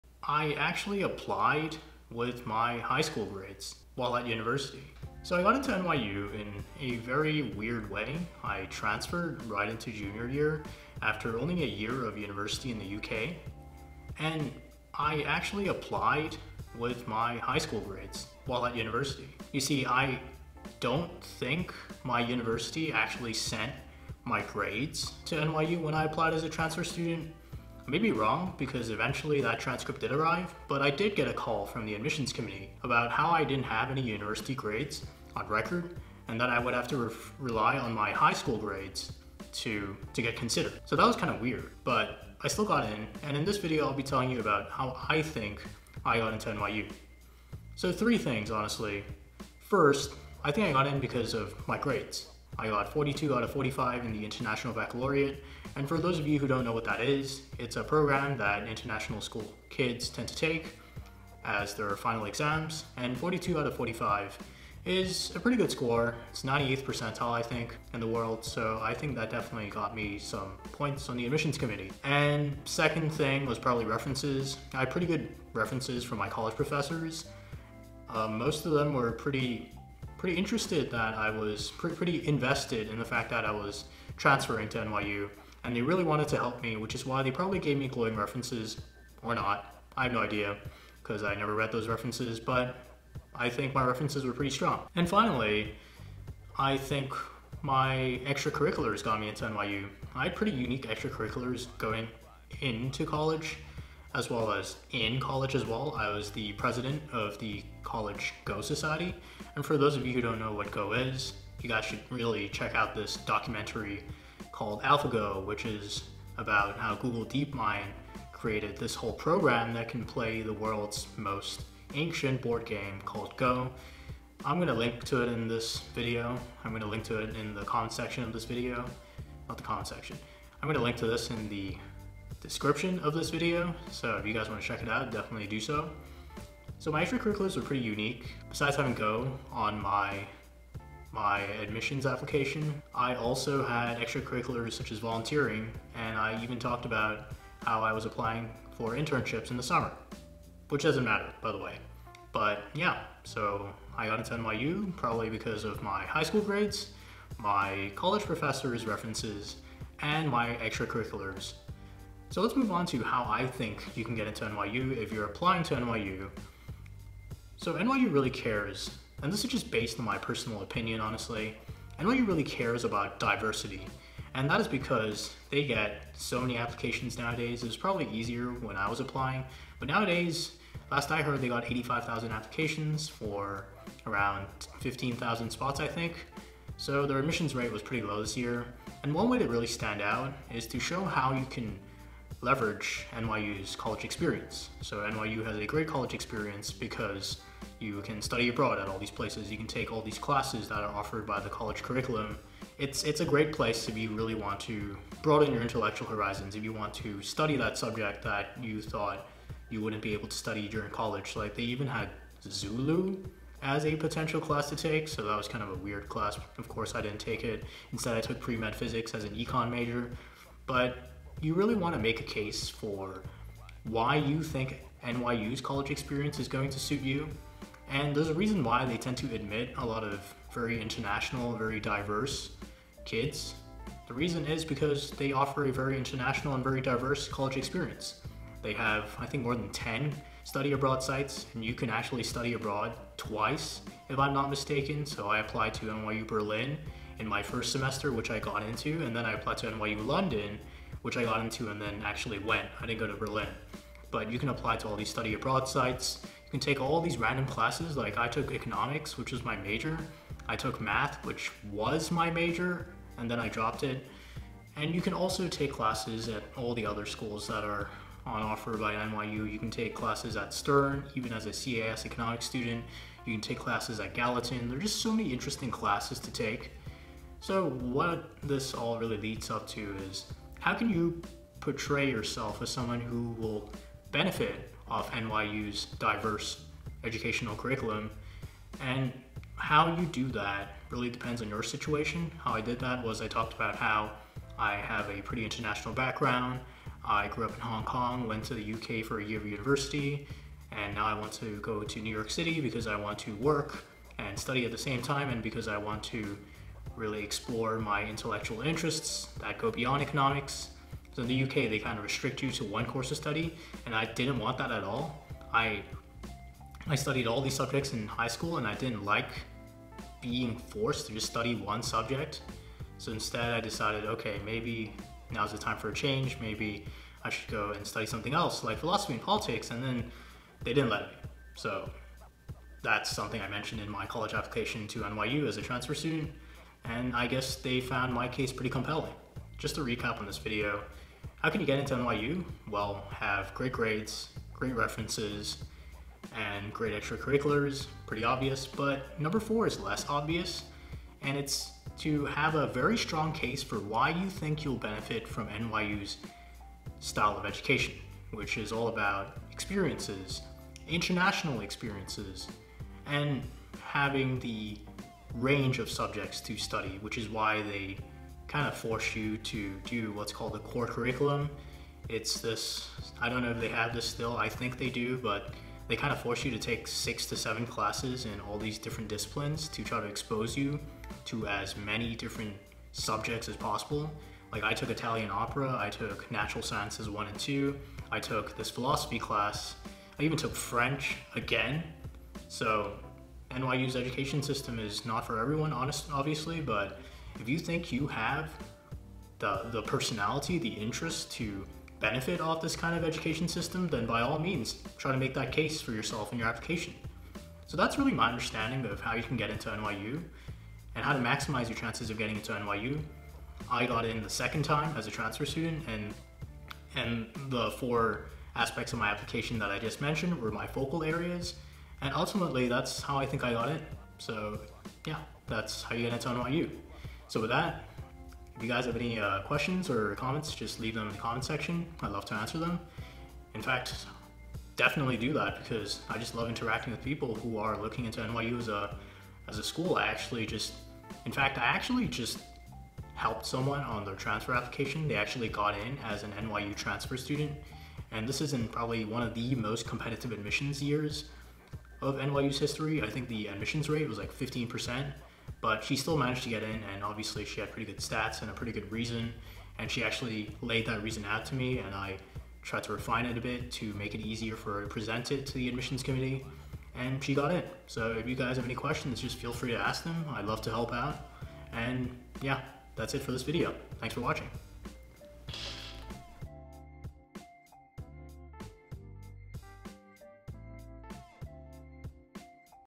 THE SECRET of How to Get Into NYU
The key to getting into NYU is to make a case for why you want to go there and what you think you'll get out of the experience.

Research NYU and the kind of college experience it offer before applying.

In some ways, NYU does not offer a traditional college experience. You'll find a lot of your friends disappearing after class to go to an event in NYC or to an internship.

Think about it from NYU's perspective.

They don't want people who won't fit into the college experience they offer.

So know what you're getting into, what you want out of a college experience and why NYU can best serve your needs.